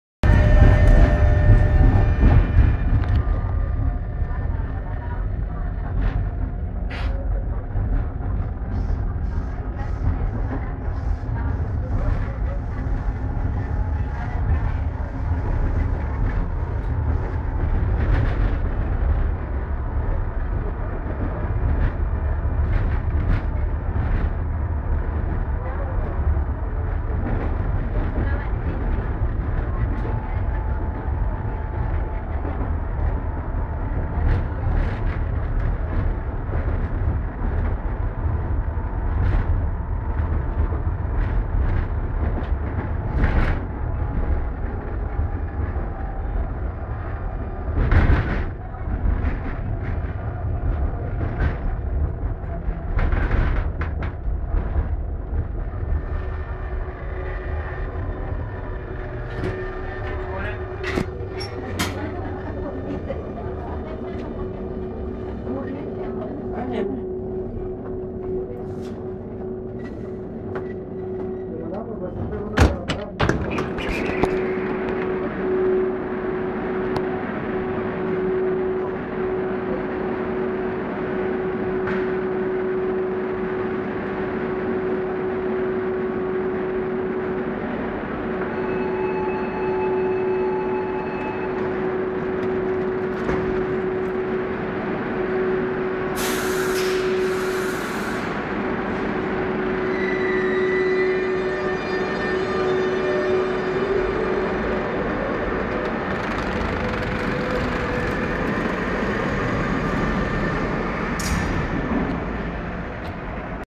Metro FGC Barcelona: trayecto entre Provença y Gràcia L6
Recorrido entre las estaciones de Provença y Gràcia de la L6 de FGC, a bordo de un tren de la serie 111.